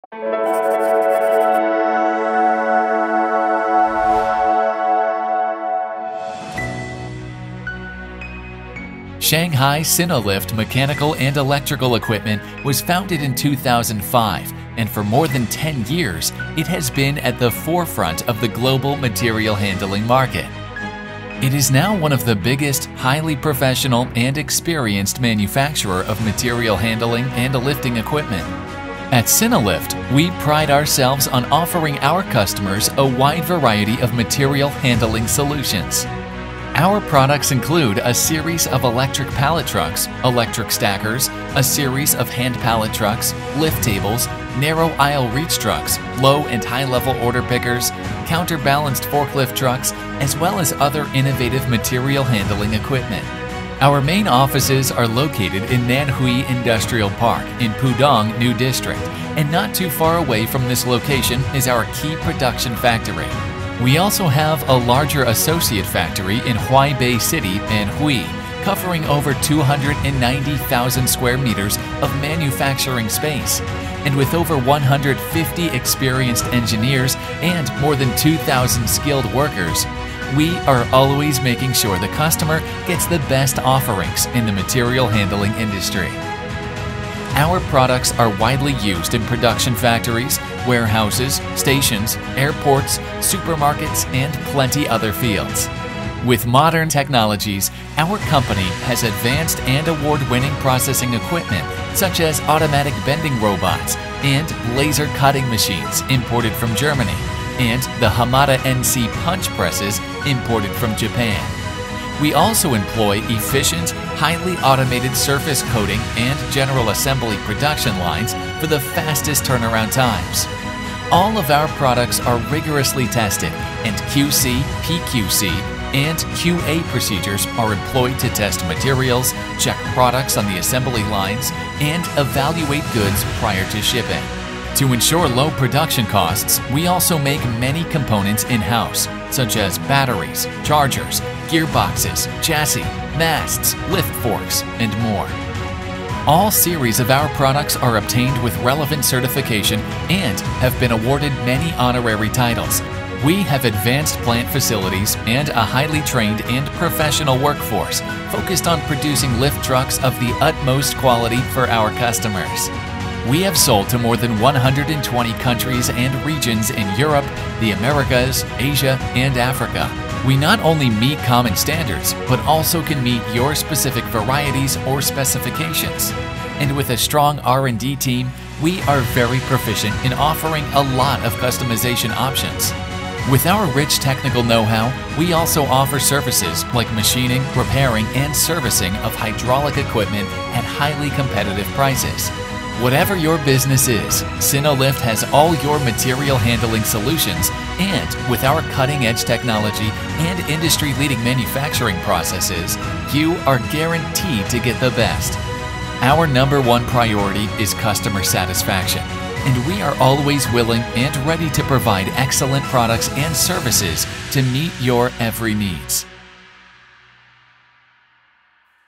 SINOLIFT -----Company Profile
Offering warehousing equipment and materials handling solutions since 2005, SINOLIFT continues to remain at the forefront of this industry. All our products meet with current CE, ISO09001, ISO14001 certification and are supplied with guarantees of 12 or 24 months.
Each machine is thoroughly tested, and spare parts are readily available. We currently offer more than 1,000 different products to suit any application.
Our factory located in Hangtou Damaiwan insustry park, Nanhui, Pudong new district, Shanghai and one associated partner factory in Huaibei City of Anhui, owns 290,942 square meters workshops, over 150 experienced engineers and more than 2000 skilled workers.
SINOLIFT enables flexibility and allows us to provide custom-made products for our customers, quickly and precisely. We will provide the most suitable solution for each objective, according to client's needs.
We offer free delivery within Shanghai, China mainland and we regularly ship worldwide.